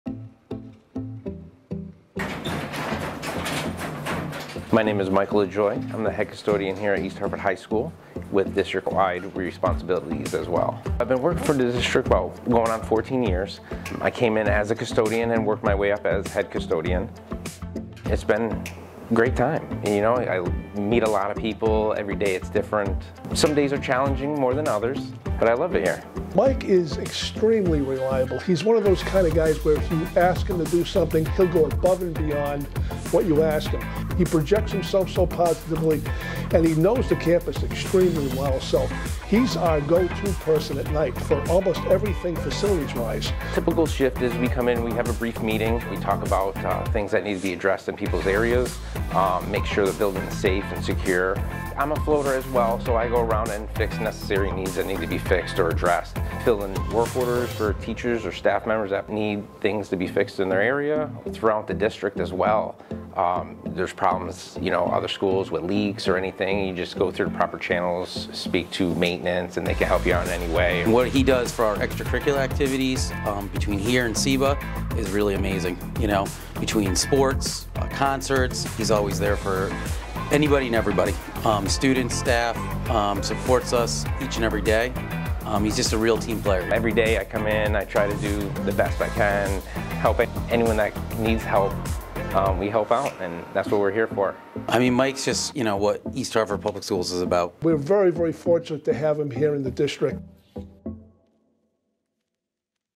EHPS STAFF SPOTLIGHT: Mike LaJoy
Mike LaJoy, an East Hartford graduate himself, is the head custodian for the EHHS and CIBA campus. Through his leadership, problem-solving abilities, and work ethic he takes great care of the buildings, in addition to the students and staff.